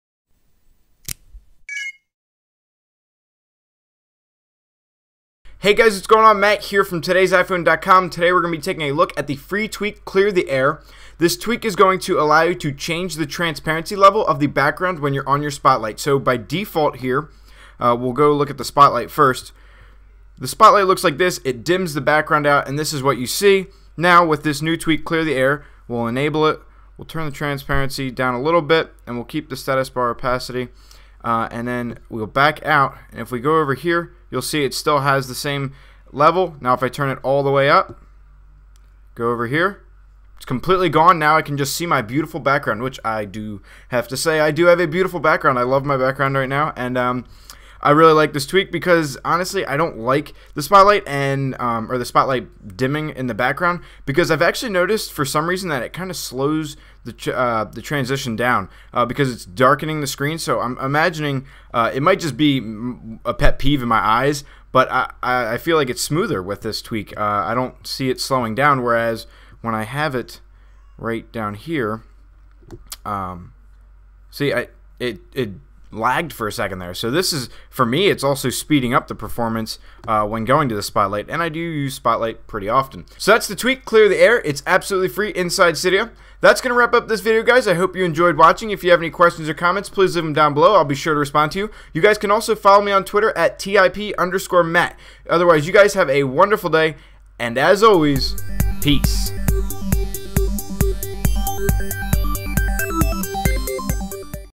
Adjust The Transparency Levels Of Your iOS Devices Spotlight Page With "Clear The Air"
Today Matt demos a tweak that allows users to adjust the transparency level of their iOS devices spotlight page.

Tweak: Clear The Air